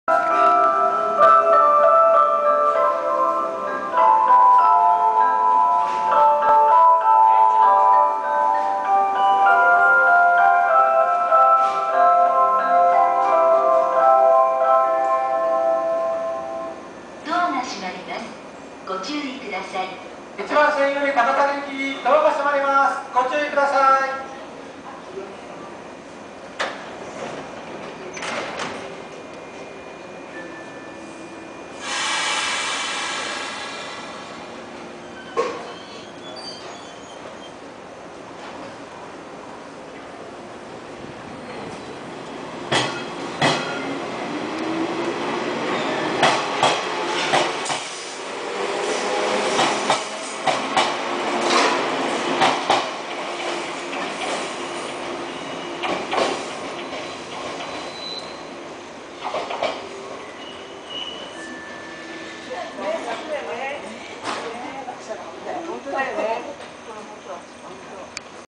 JR東日本 上越線115系 発車＠水上(始発)
※2015年撮影。
水上駅を発車する「普通 高崎行き」です。
冒頭から発車メロディが流れています。